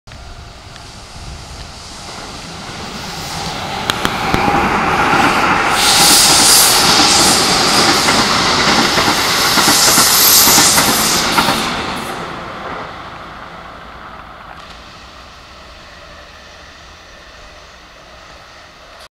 京急線追浜駅手前の踏切付近を、下り8両編成快特三崎口行き2100形17～24編成の電車が、次の停車駅横須賀中央駅に向けて走行中です！
京急線追浜駅手前の踏切付近から撮影！
金沢八景駅を発車して追浜駅を通過し、次の停車駅停車駅横須賀中央に向けて走行中の下り８両編成快特三崎口行き2100形17～24編成の電車が踏切を通過して走行して行きます！